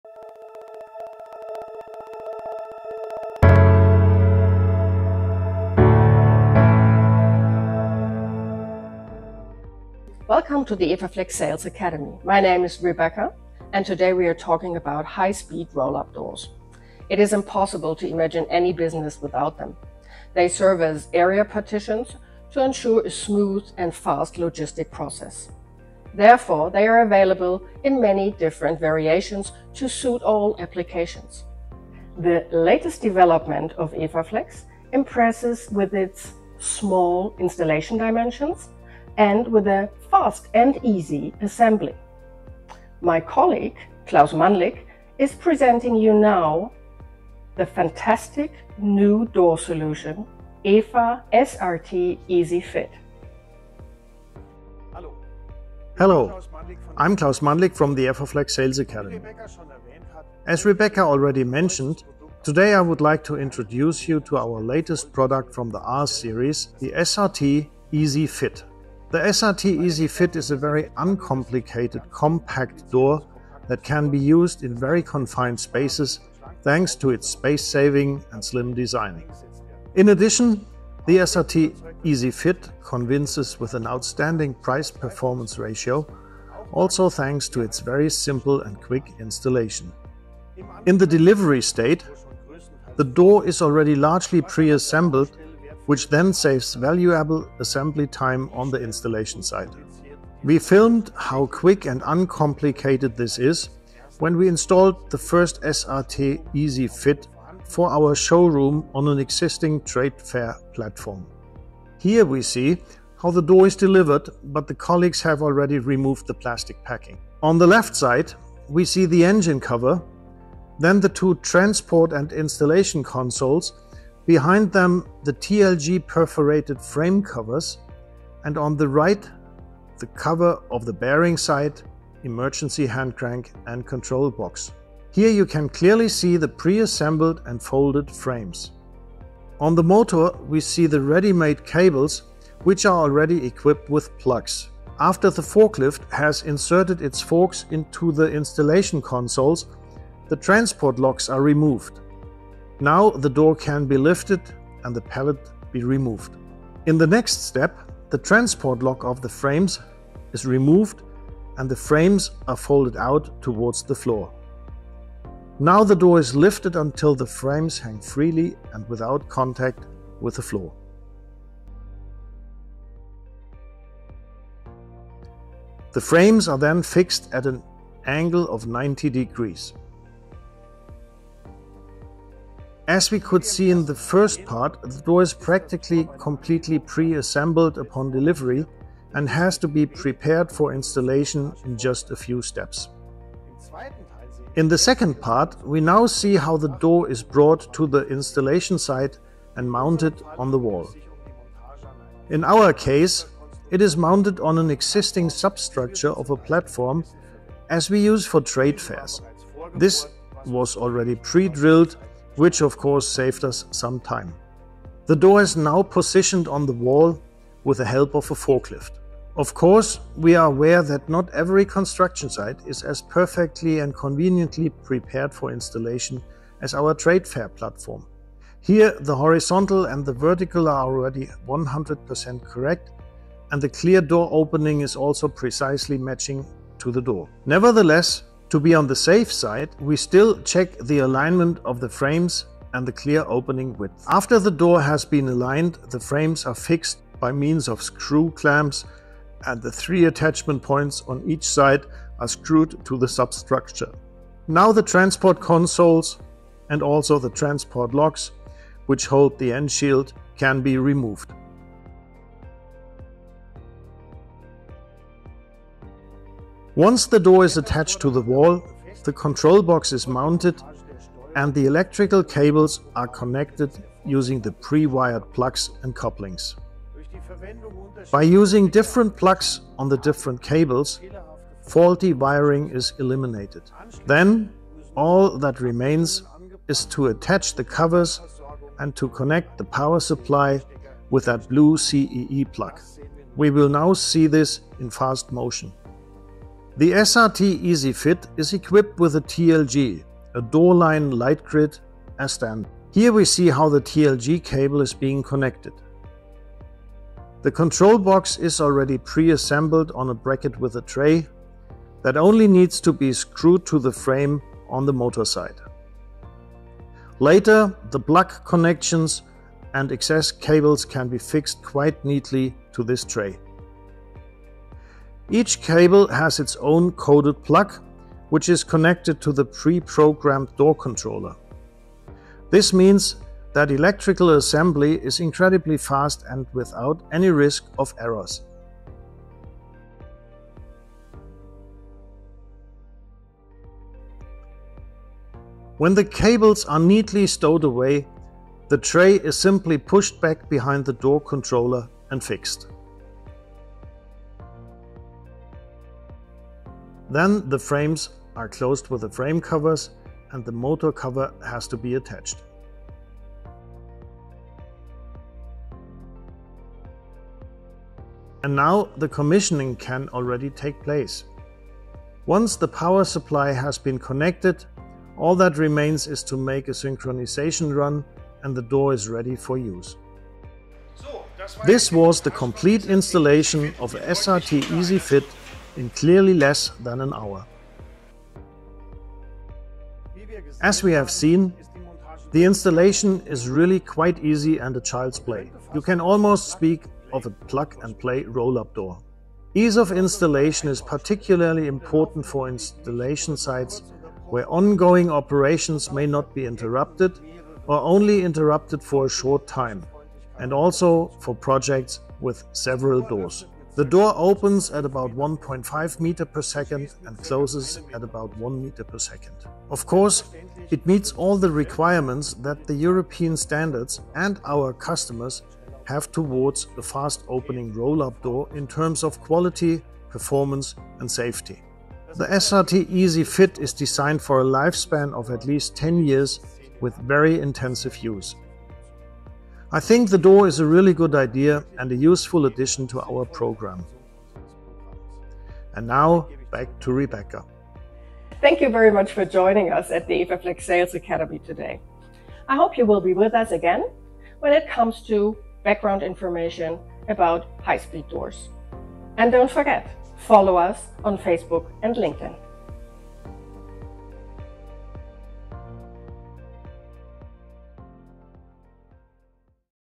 The new EFA-SRT®Easyfit
Discover the latest tutorial from EFAFLEX! In this video, we demonstrate the simple installation process of the EFA-SRT® EasyFit. Experience how this high-speed door impresses not only with its speed but also with its outstanding price-performance ratio. The EFA-SRT® EasyFit opens at a speed of 1.5 meters per second and closes at 1 meter per second – perfect for efficient and rapid operations. Watch the video for detailed installation instructions and learn more about the benefits of this innovative high-speed door.